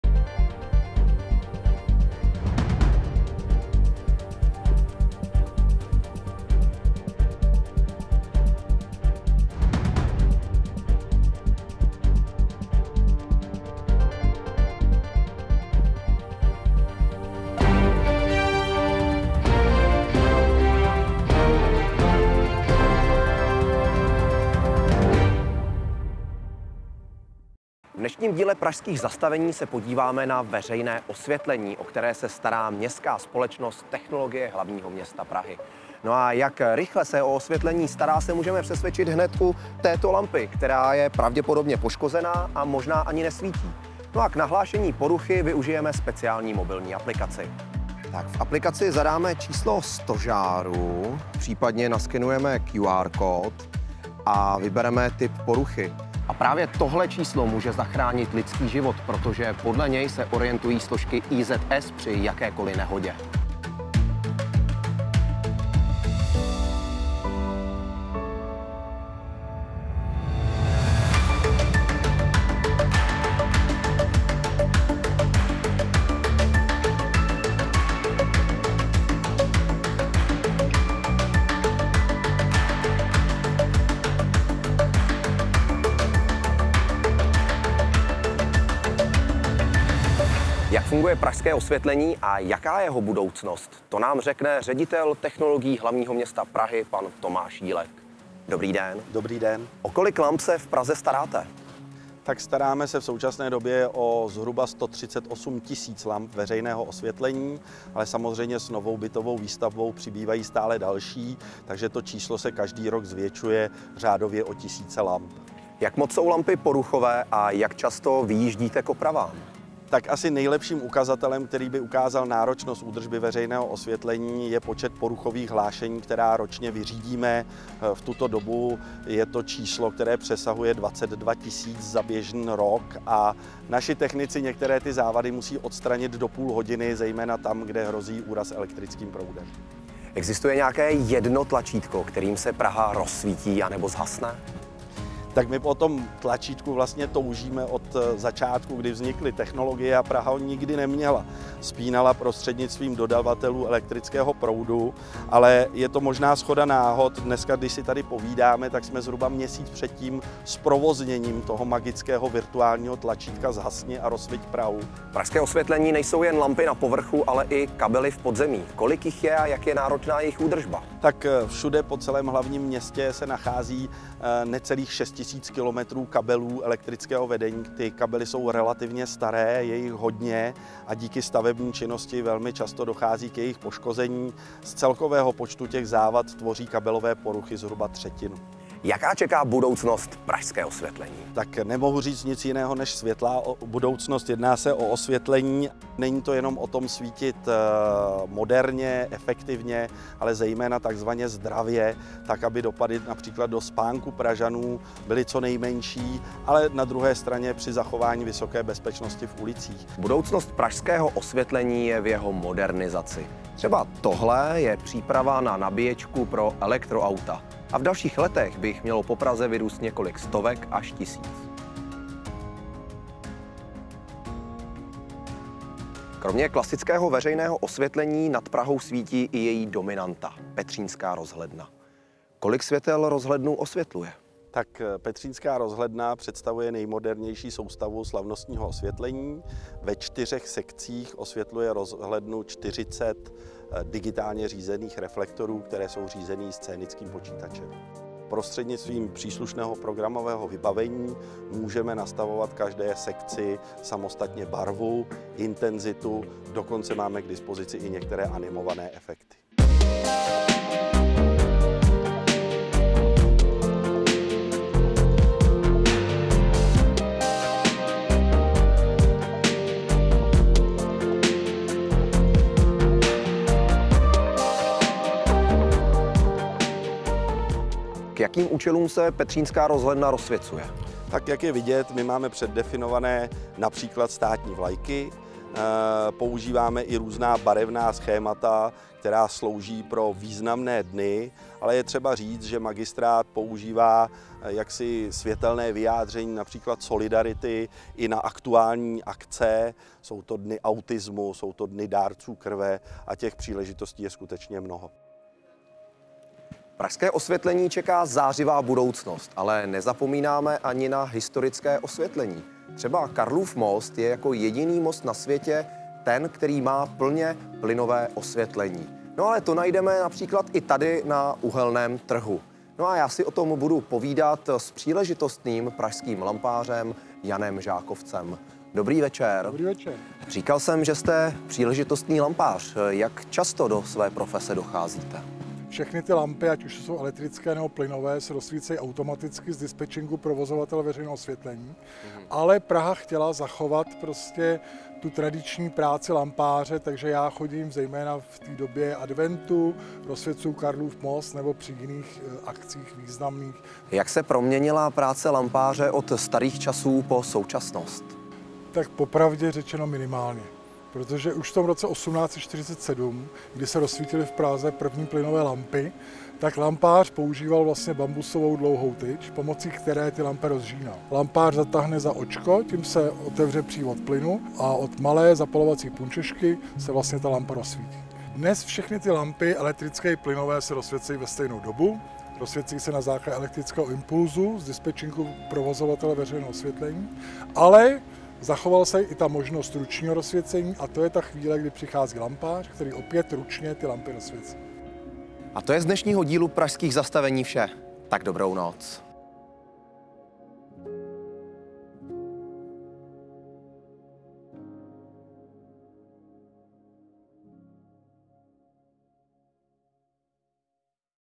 Pražská zastavení - Osvětlení ve městě (české titulky)
Podívejte se v novém díle Pražských zastavení, jakým způsobem se Praha stará o 138 000 lamp veřejného osvětlení, ke kterým vede 6 000 kilometrů kabelů. Moderní osvětlení přitom v historickém centru doplňují i historické plynové lampy. V novém díle se také mimo jiné dozvíte, jak poruchové je veřejné osvětlení a jak často se opravuje, jaké barvy může mít rozhledna, i to, kdy Prahu obchází lampář zapalující plynové lampy.